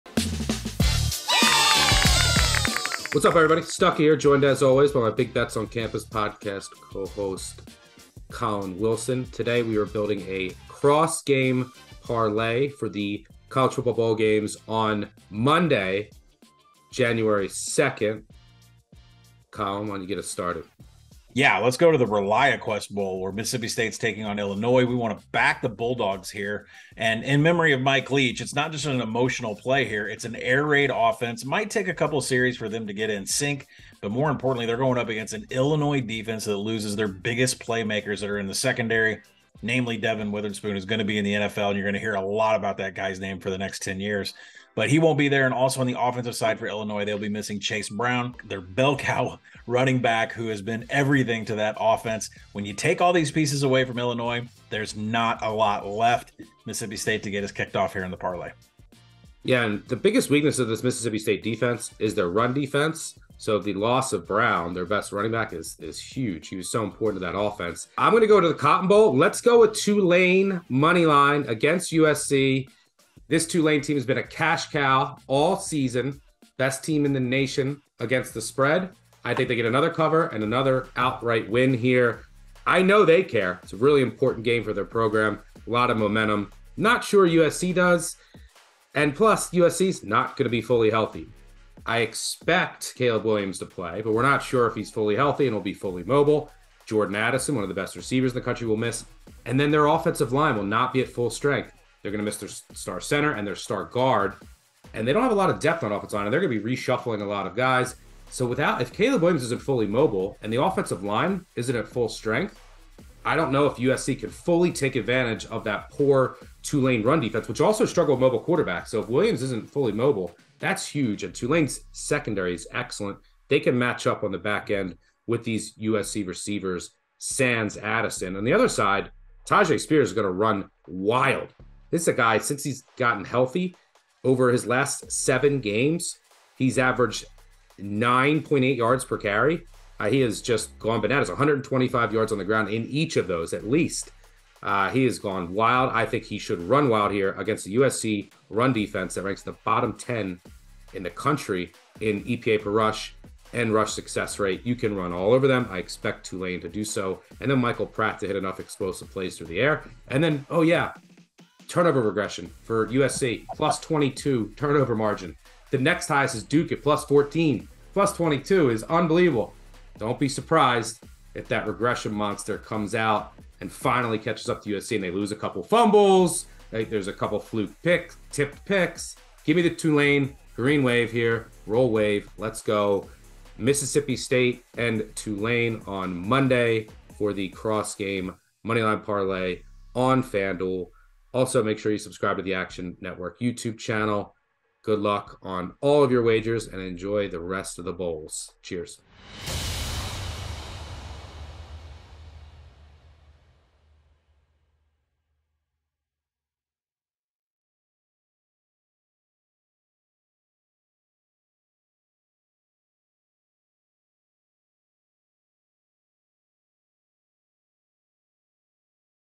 College Football Bowl Game Parlay 01/02 | Mississippi St vs Illinois & Tulane vs USC Picks
College Football betting experts Stuckey and Collin Wilson build a bowl game parlay using picks from Mississippi State vs Illinois and Tulane vs USC.